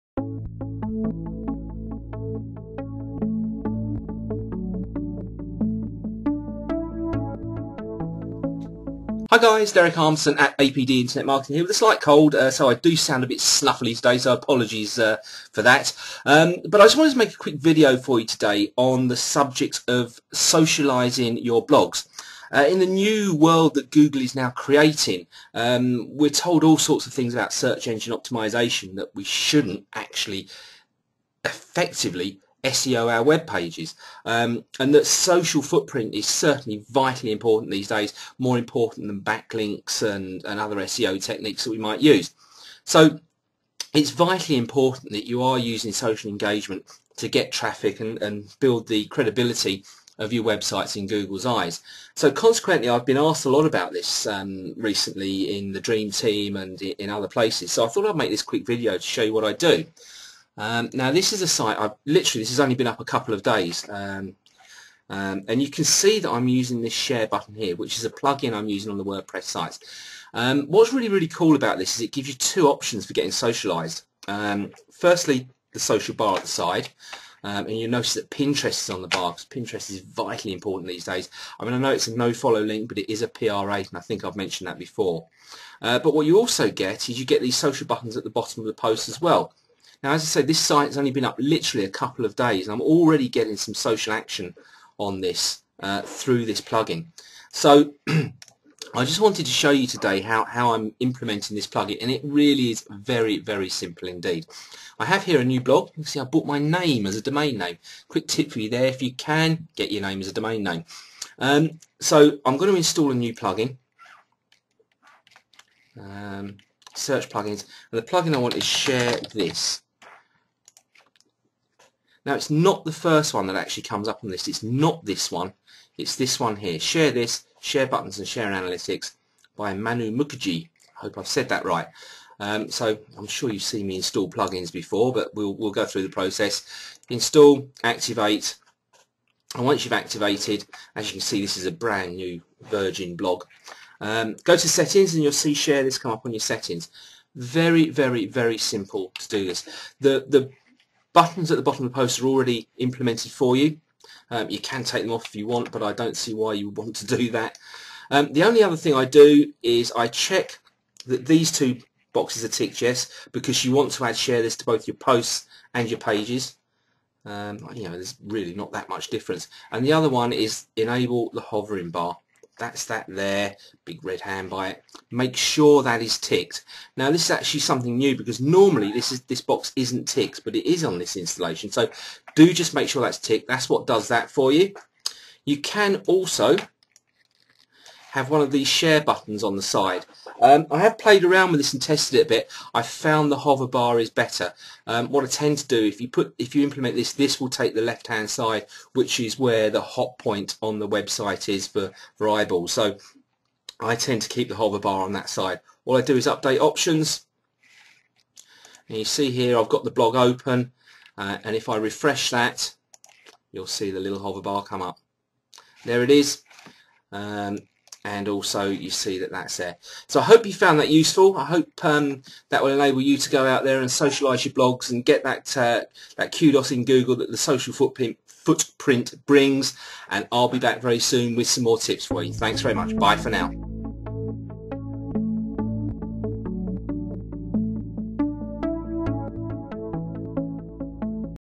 Social Media SEO - How To Get Social Buttons On Your Wordpress Blog
Hi Guys,

Derek Armson at APD Internet Marketing here with a slight cold so I do sound a bit snuffly today so my apologies for that. I just wanted to make a quick video for your today on the subject of socialising your blogs.

In the new world that Google is now creating, we are told all sorts of things about Search Engine Optimization, that we shouldn't actually effectively SEO our webpages. And, that social footprint is vitally important these days, more important than backlinks and other SEO techniques that we might use.

So, it's vitally important that you are using social engagement to get traffic and build the
credibility of your websites in Google's eyes. Consequently I have been asked a lot about this recently in the Dream Team and other places.

So, I thought I'd make this quick video to show you what I do.

Now, this is a ste that has literally only been up a couple of days and you can see that I am using this share button here which is a plugin I am using on my Wordpress sites. What's really, really cool about this is that it gives you two options for getting socialized.

Firstly the social bar at the side, and you'll notice Pinterest is on the bar as Pinterest is
vitally important these days. I know it's a no follow link but it is a PR 8 link, I think I've
mentioned that before.

But what you also get is you get these social buttons at the bottom of the post as well.

As I said this site has only been up literally a couple of days and I'm already getting some social action on this, through this plugin.

So, I just wanted to show you today how I'm implementing this plugin and it really is very, very simple indeed.

I have here a new blog, as you can see I've bought my name as a domain name - quick tip for you there, if you can get your name as a domain name do it.

I am going to install a new plugin.

Search plugins and the plugin I want is called share this. Now it's not the first one that
actually comes up in this list, it's not this one...it's this one here. Share This - Share
Buttons and Analytics by Manu Mukerji - I hope I said that right.

I'm sure you have seen me install plugins before, but we'll go through the process.

Install, activate and once you've activated, as you can see this is a brand new virgin blog, go to settings and you'll see share this come up on your settings.

Very, very, very simple to do this.

The buttons at the bottom of the post are already implemented for you - you can take them off if you want but I can't see why you would want to do that? The only other thing I do, is I check that these two boxes are ticked "yes" because you want to add sgare this to both your posts and pages - there's eally not that much difference.

And the other one is "enable the hovering bar" - its that there, big red hand by it. Make sure that is ticked. This is something new because this box is not normally ticked by default, but it is on this installation, so do just make sure this is ticked. This is what makes the bar "come up" for you.

You can also have one of these share buttons on the side. I have played around with this and tested it a bit but I have found the hover bar is better. What I tend to do, because if you implement this, this will take the left hand side which is where the hot point is on the website for eyeballs. So I tend to keep the hover bar on that side.

All I do is update options. You see here I've got the blog open and if I refresh that you'll
see the little hover bar come up. There it is. And also you see that, that's there (the bottom social share buttons).

So, I hope you found that useful, I hope that will enable you to go out there and socialize your blogs and get that kubos in Google that the social footprint brings. I'll be back very soon with some more tips for you.